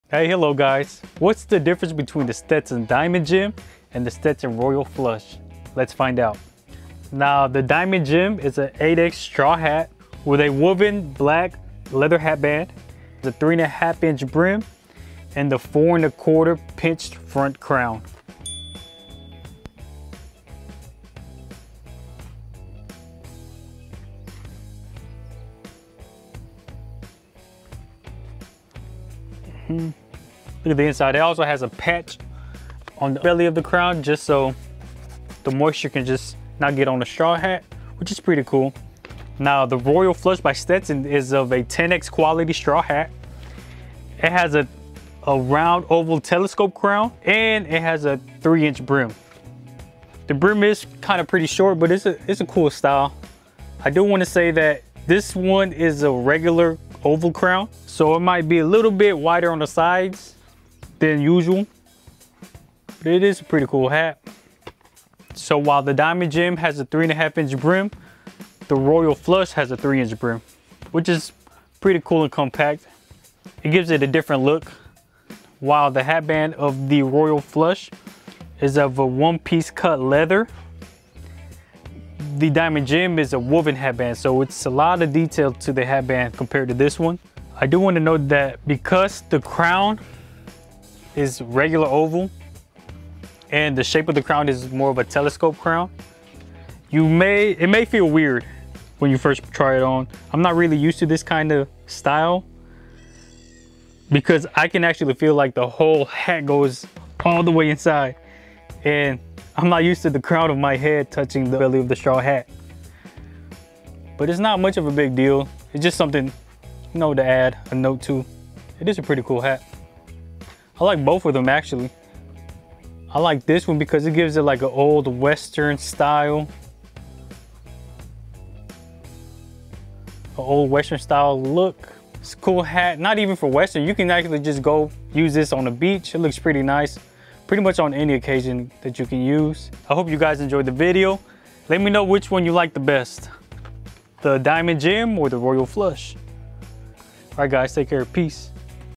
Stetson Straw Hats Diamond Jim x Royal Flush
Here I have two Stetson straw hats that are pretty popular this time of year. Great for summer gatherings and adventures.